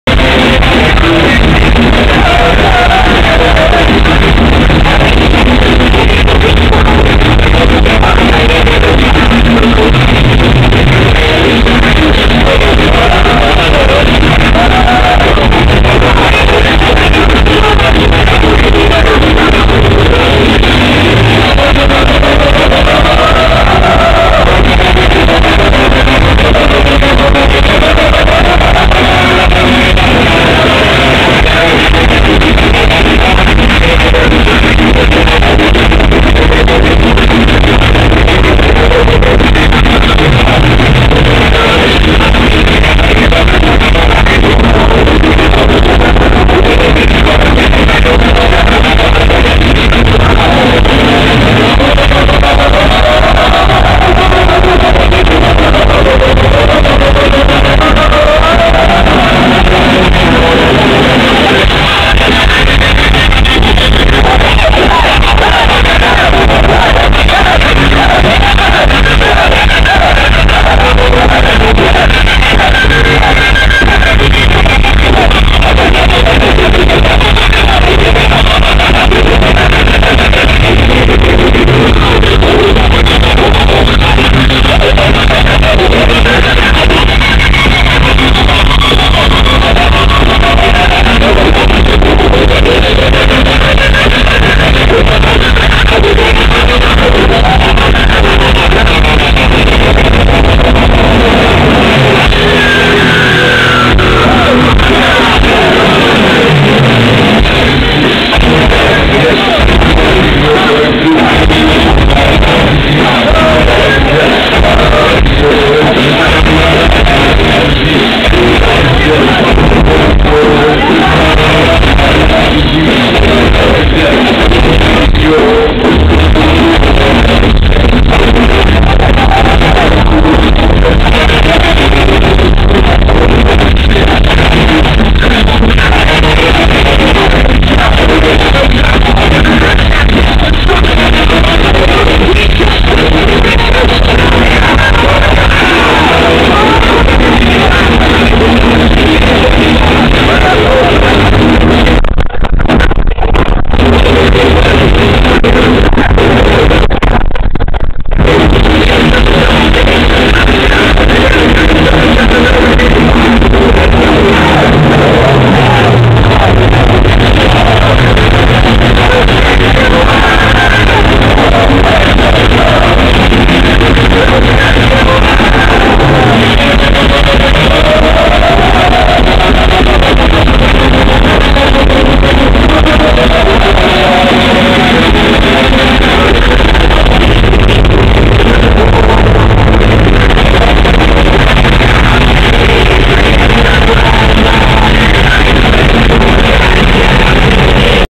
Y asi empezo SLAYER.Argentina 08/11/12 (pogo)
Bueno el video se escucha para el orto jaja,encima filmado desde el pogo =D..Maquinaria Fest